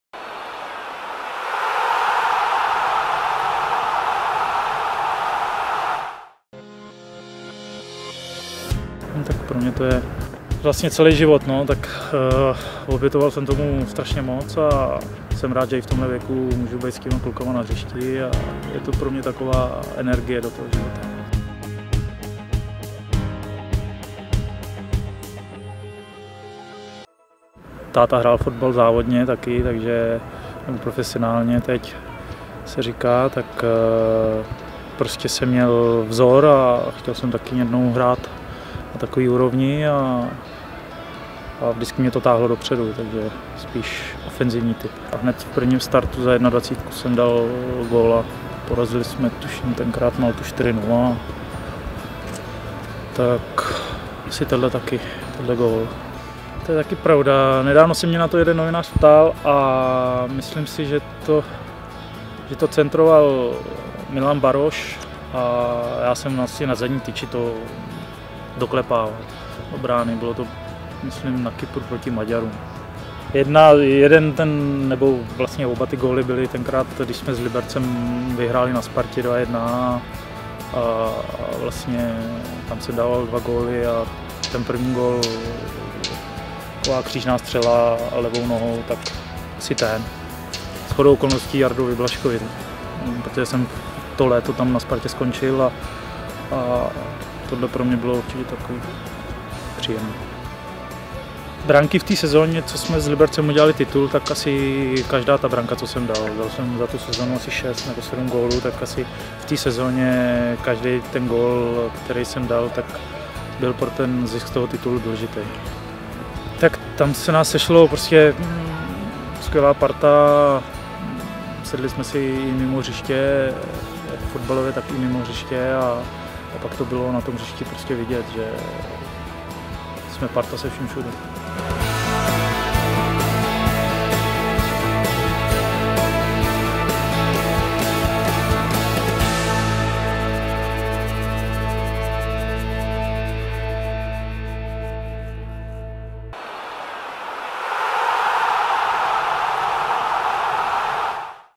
Václav Koloušek: Fotbal mi dává energii do života
Je dvojnásobným ligovým mistrem a v samostatné české nejvyšší soutěži oblékal dresy Sparty, Slavie, Liberce, Příbrami a také jihlavské Vysočiny. Když se řekne jeho jméno, fotbalový fanoušek si hned vybaví silnou stránku jihlavského špílmachra. „Přímé kopy a střely ze střední vzdálenosti, to bylo moje, usmívá se zkušený jihlavský záložník.

MŮJ PRVNÍ GÓL

Dejte LAJK naším videím či rovnou odebírejte náš kanál a žádný první gól vám neunikne.